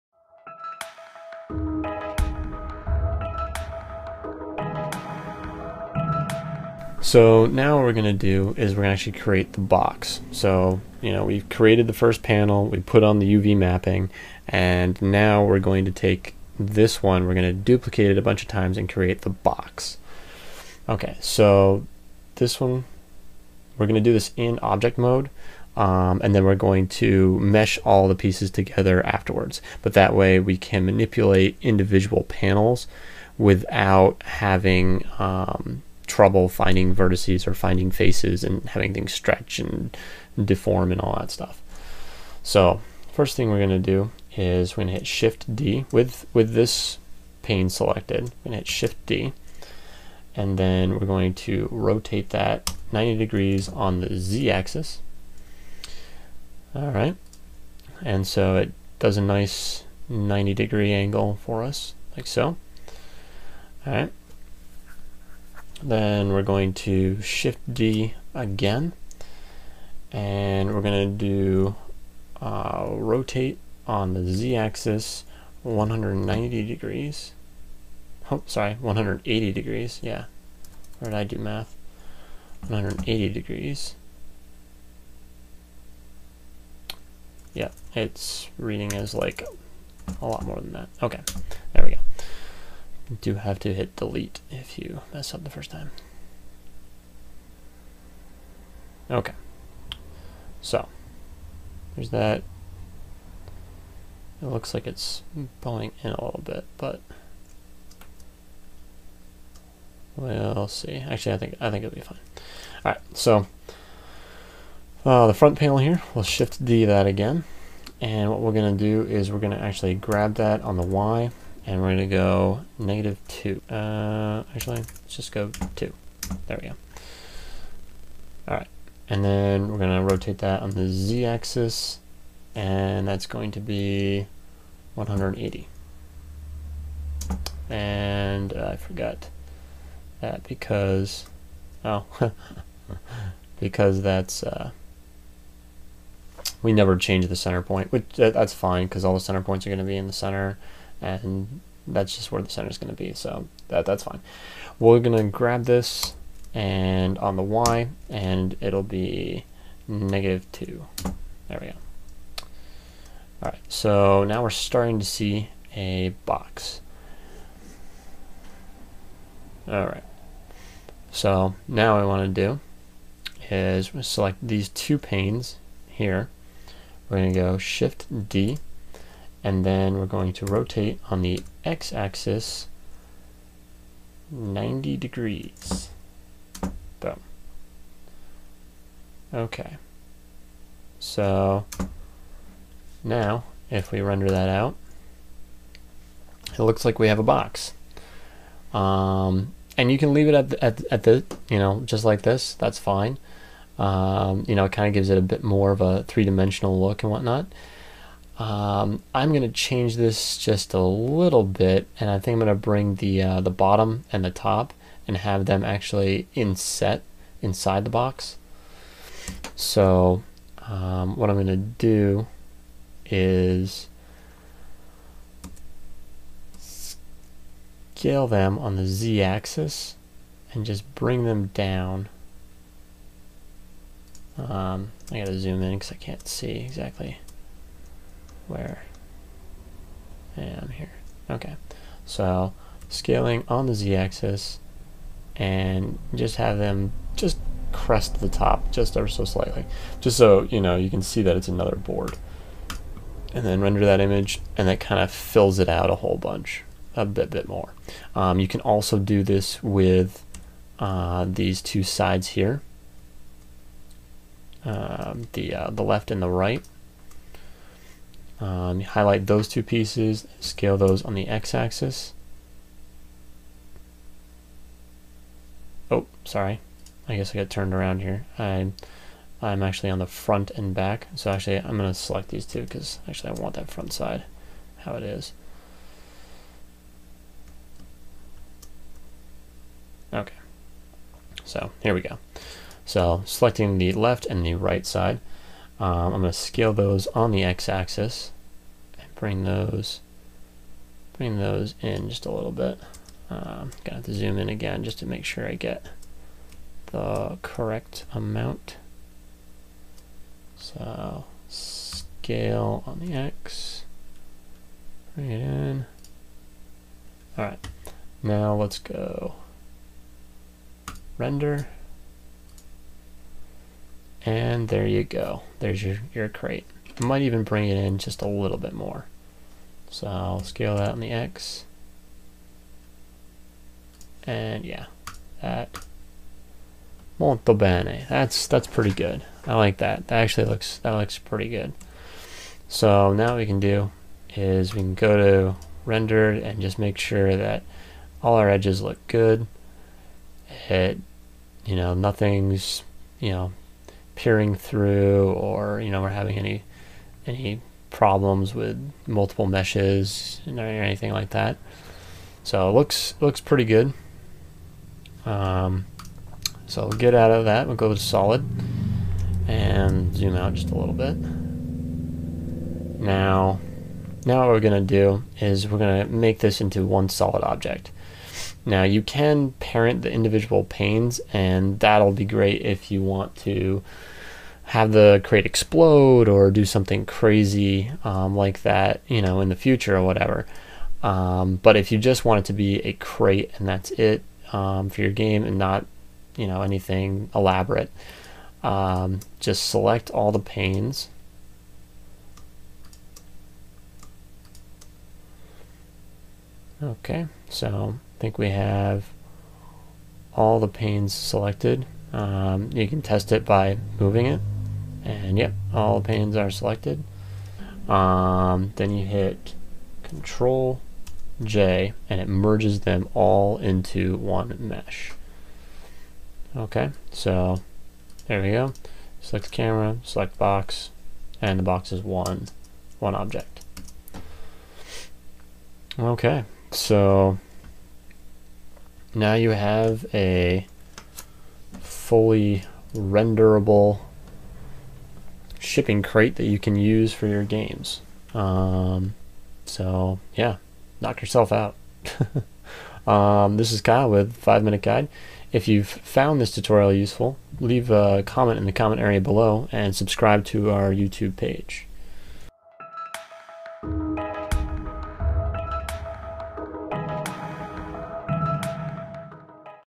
Blender Tutorial - Game Elements - Shipping Crate Part 3
In this tutorial we take the side of the shipping crate that we already uv wrapped and make it into a crate. one solid object you can throw into your games.